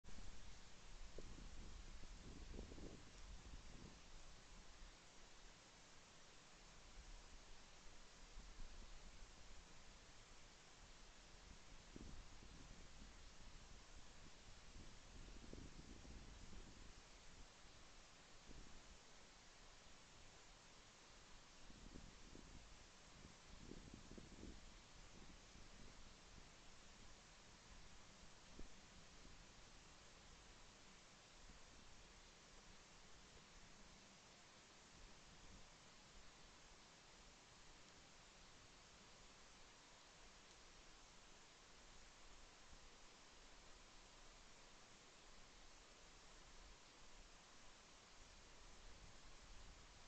Cervo 🦌 - Cervus elaphus
Videotrappolaggio di un Cervo maschio in primo piano davanti alla fototrappola in una zona panoramica del Parco Nazionale Foreste Casentinesi